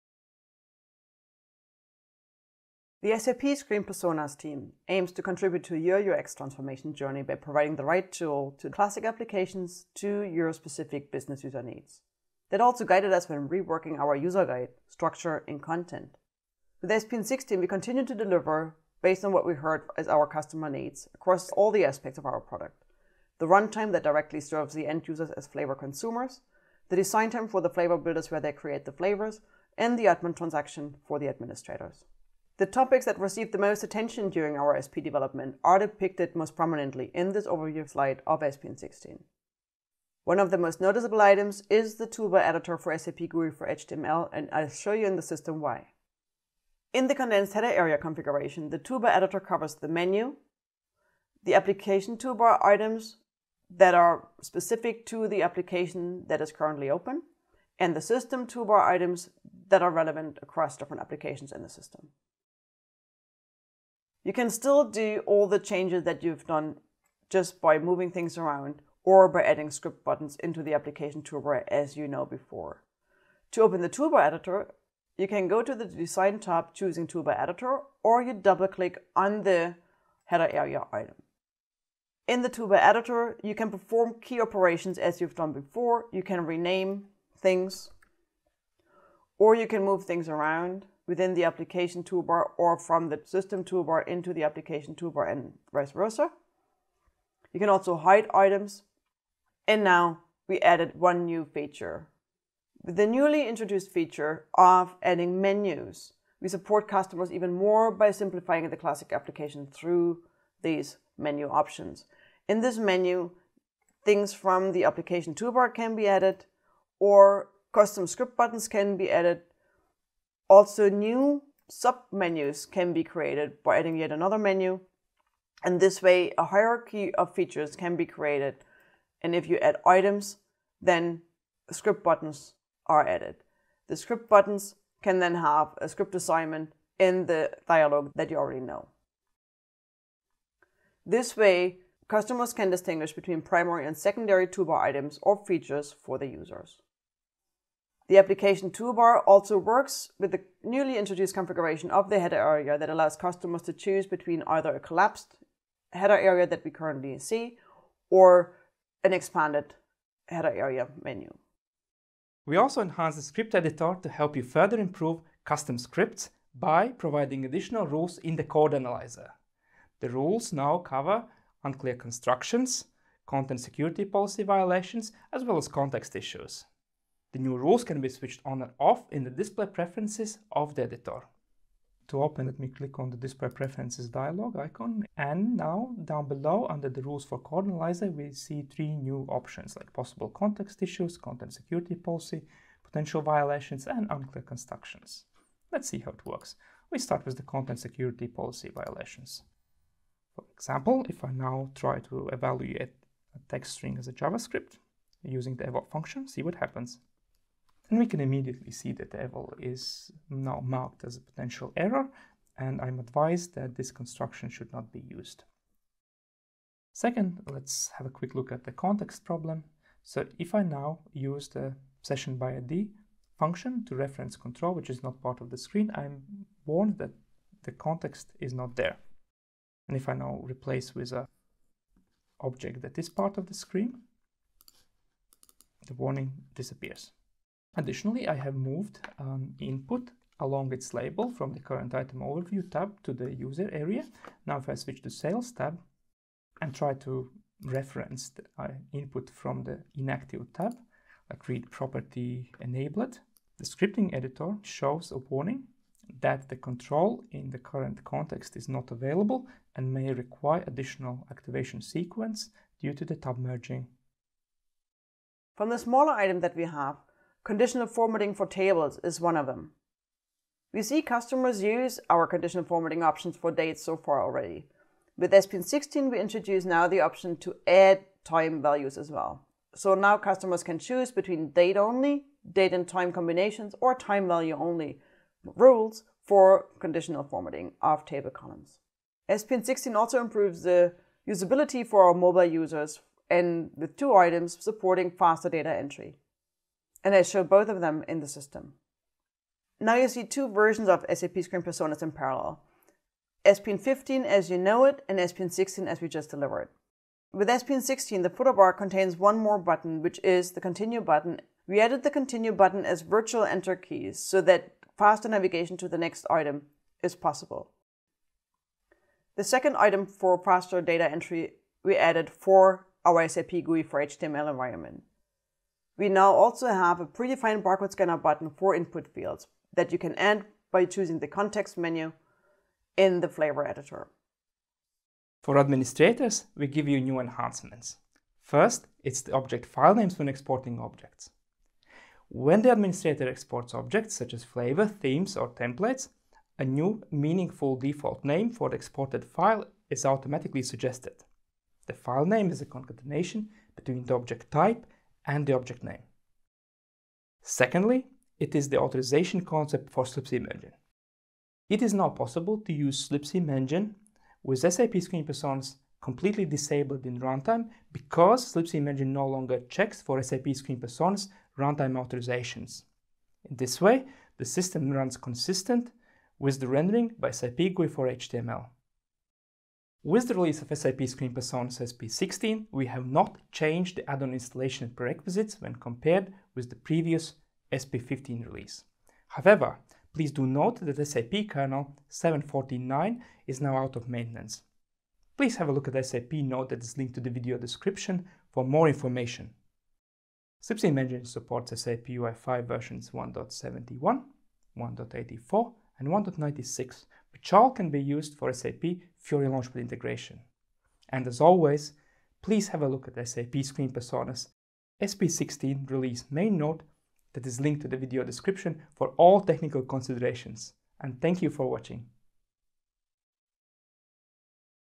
SAP Screen Personas SP16 Highlights
Katrin Polloczek and Krists Magons of the SAP Screen Personas team demonstrate new features in SAP Screen Personas SP16. Topics covered include the toolbar editor, code analyzer, and enhancements to mobile runtime and administration.